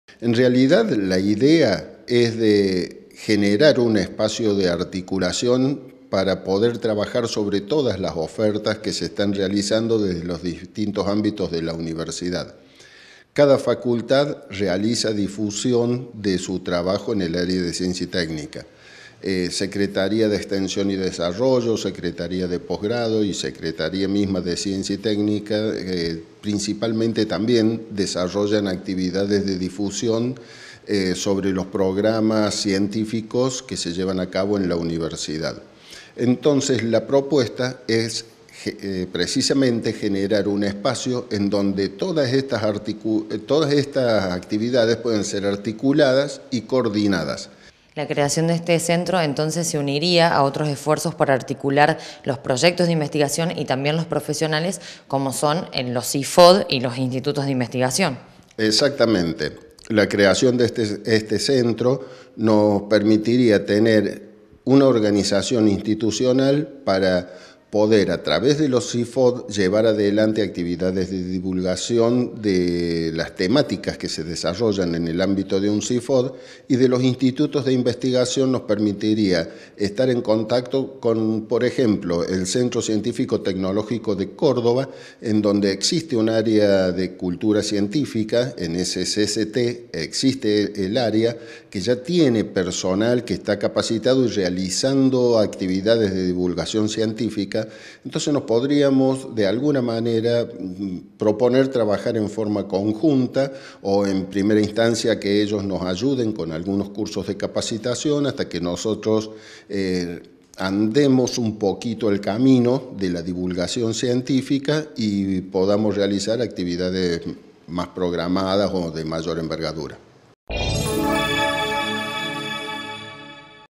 CENTRO DE CULTURA CIENTÍFICA - JUAN MARIOLI – SECRETARIO DE CIENCIA Y TÉCNICA
EL CONSEJO SUPERIOR APROBÓ SU CREACIÓN EN LA UNRC
Facebook.com/uniriotv
Twitter.com/uniriotv